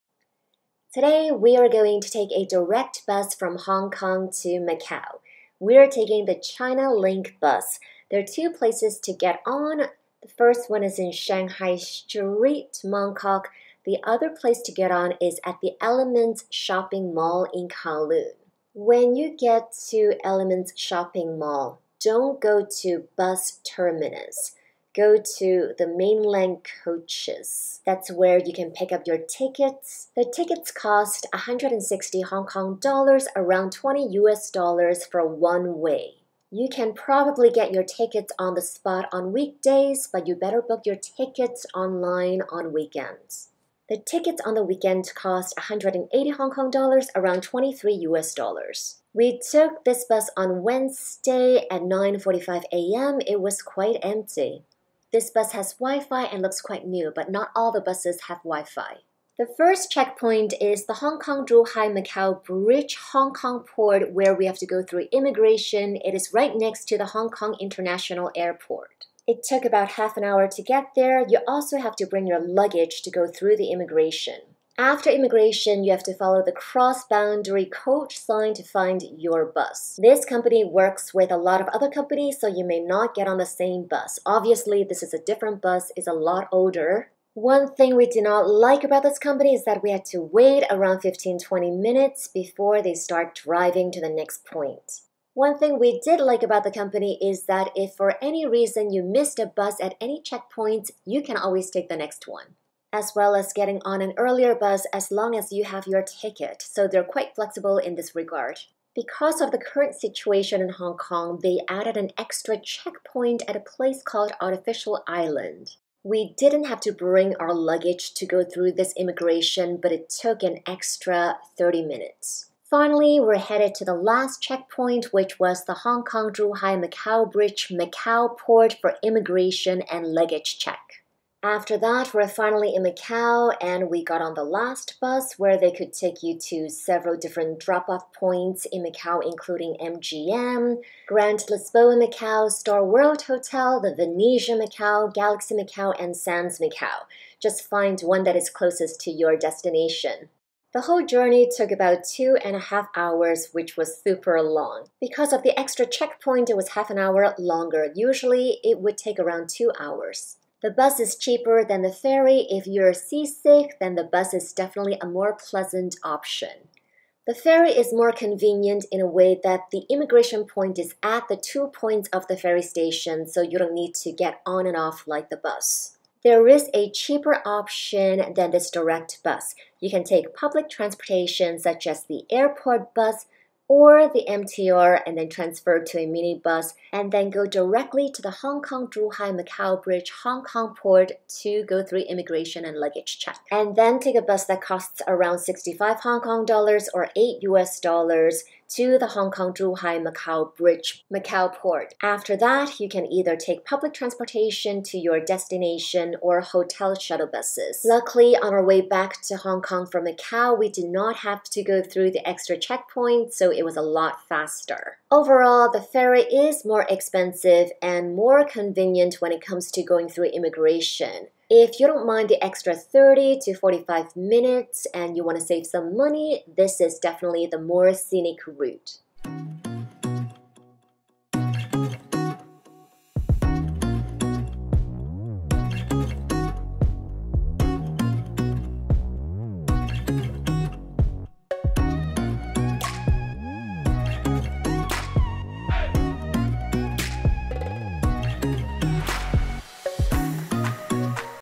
How to travel from Hong Kong to Macau by bus - Chinalink bus
Is taking the bus or the free to Macau from Hong Kong better? Find out!